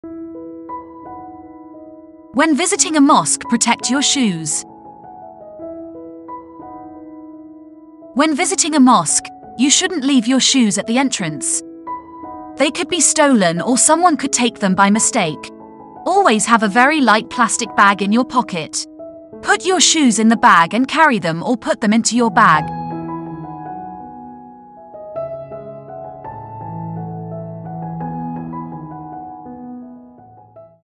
When Visiting a Mosque Protect Your Shoes
[EN] When Visiting a Mosque Protect Your Shoes.

[AR] عند زيارتك للمسجد ضع حذائك في كيس بلاستيكي خفيف جداً واحمله أو ضعه في حقيبتك

[DE] Wenn Sie eine Moschee besuchen, packen Sie Ihre Schuhe in eine sehr leichte Plastiktüte und tragen Sie sie oder stecken Sie sie in Ihre Tasche.

[ES] Cuando visite una mezquita Coloque sus zapatos en una bolsa de plástico muy liviana y llévelos o póngalos en su bolso.

[FR] Lorsque vous visitez une mosquée Mettez vos chaussures dans un sac en plastique très léger et transportez-les ou mettez-les dans votre sac.

[IND] मस्जिद जाते समय अपने जूतों को एक बहुत हल्के प्लास्टिक बैग में रखें और उन्हें अपने साथ ले जाएं या अपने बैग में रख लें

[IR]هنگام بازدید از مسجد کفش های خود را در یک کیسه پلاستیکی بسیار سبک قرار دهید و آنها را حمل کنید یا داخل کیف خود قرار دهید.

[IT] Quando visiti una moschea Metti le scarpe in un sacchetto di plastica molto leggero e portale con te o mettile nella borsa.

[PAK] مسجد میں جاتے وقت اپنے جوتوں کو ایک بہت ہلکے پلاسٹک کے تھیلے میں رکھیں اور لے جائیں یا اپنے بیگ میں رکھیں

[RU] При посещении мечети положите обувь в очень легкий полиэтиленовый пакет и носите ее с собой или положите в сумку.

When visiting a mosque, you shouldn't leave your shoes at the entrance. They could be stolen or someone could take them by mistake.

Always have a very light plastic bag in your pocket. Put your shoes in the bag and carry them or put them into your bag. Otherwise shoes are not allowed to be taken inside without being placed in a bag.